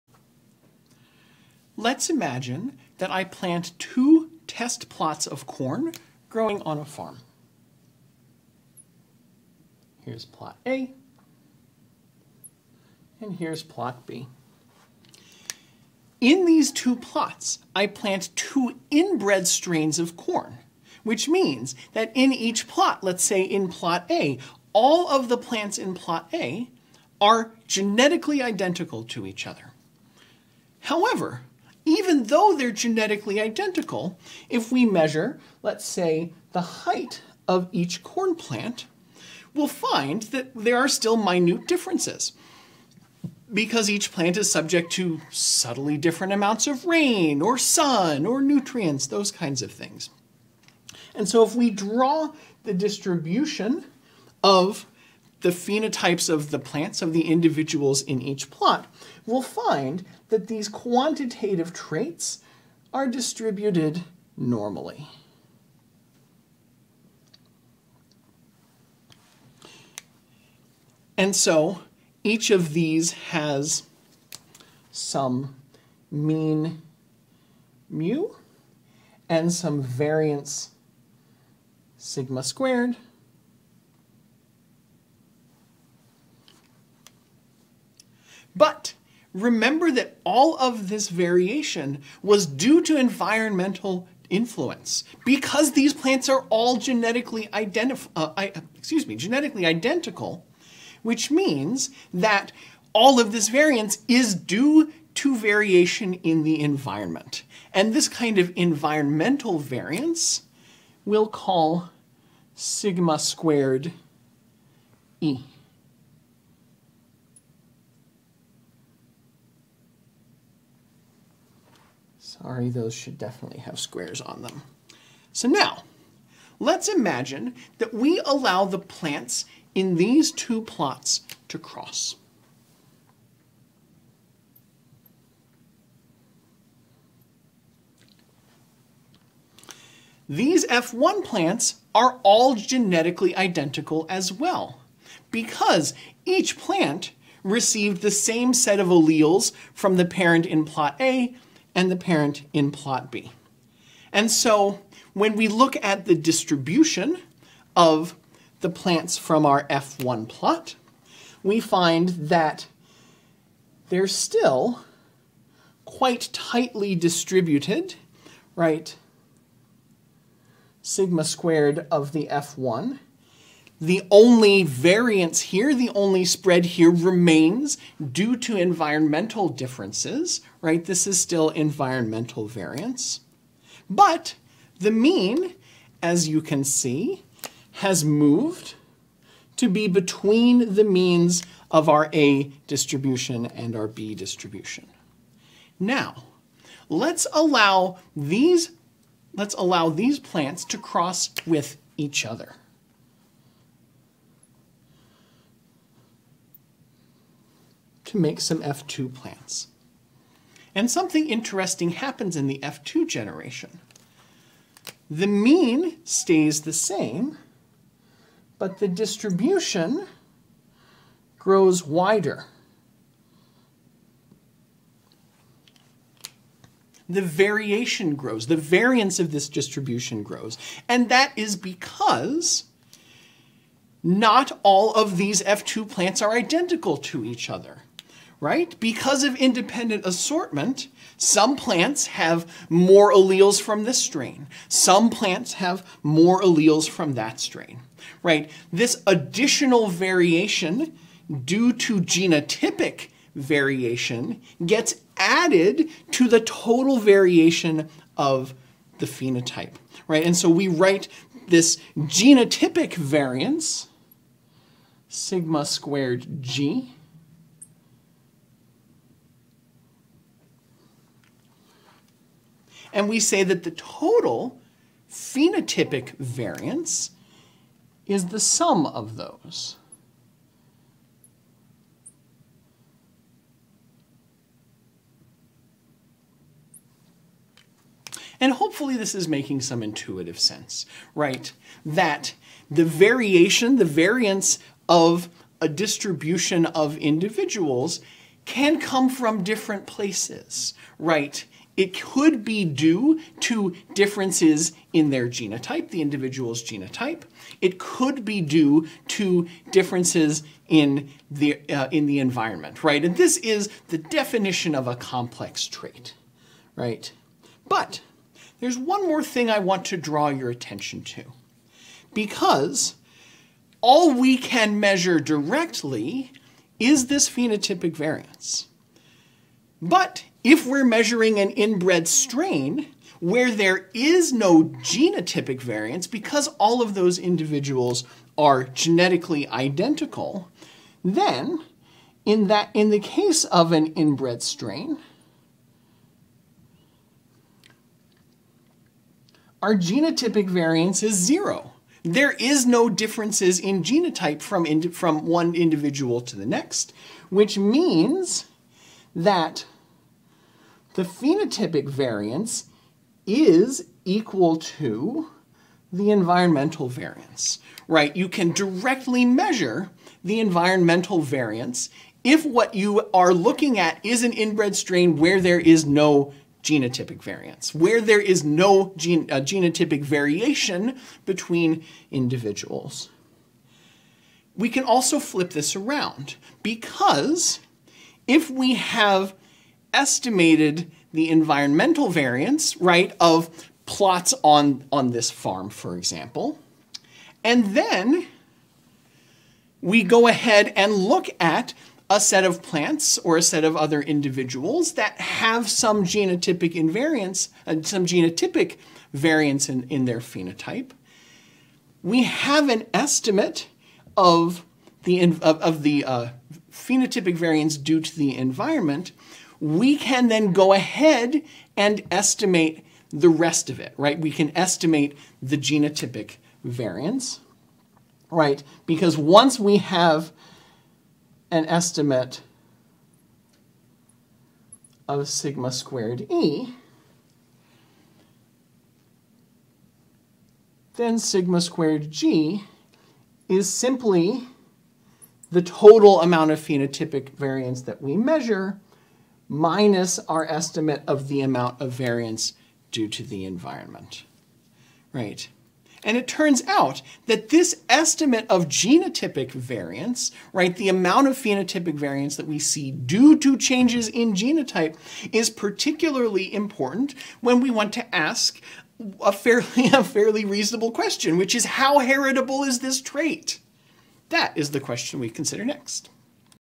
What causes variation in quantitative traits?
What is environmental variance? What is genotypic variance? What is phenotypic variance, and how is it related to the environmental variance and genotypic variance in a population? How can we use an inbred strain to directly measure environmental variance?

This video lecture was recorded at the University of Wisconsin -- Stout in the fall of 2021. It accompanies my BIO 332 Genetics course, which was taught with Hartl's "Genetics: Analysis of Genes and Genomes."